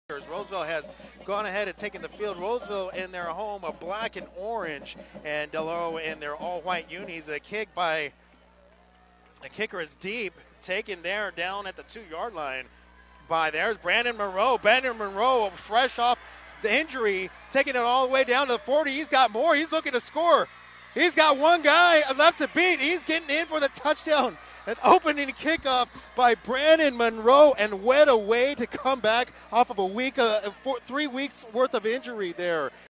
Del Oro KR #32 Brandon Monroe 98 Yard Opening Kickoff Return TD
Monroe takes the opening kickoff for a 98 yard TD as Del Oro takes the early 6-0 lead over Roseville in this CIF Sac-Joaquin Section contest.